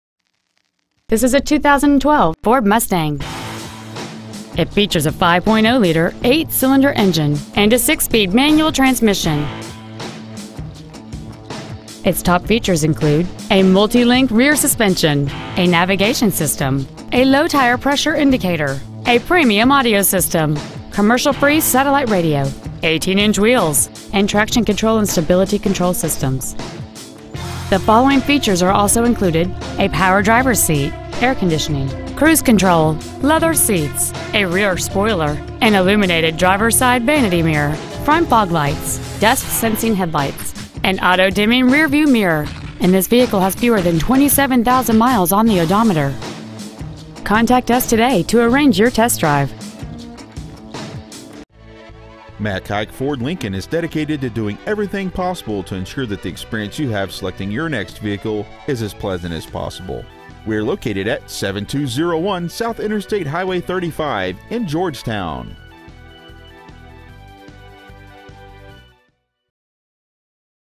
Preowned 2012 FORD MUSTANG Georgetown TX
Year : 2012
 Make : FORD
 Model : MUSTANG
 Engine : 5.0L V8 32V MPFI DOHC
 Trans . : 6-SPEED MANUAL
 Exterior : GRABBER BLUE
 Miles : 26,431
 Interior : CHARCOAL BLACK
 Stock : 130531A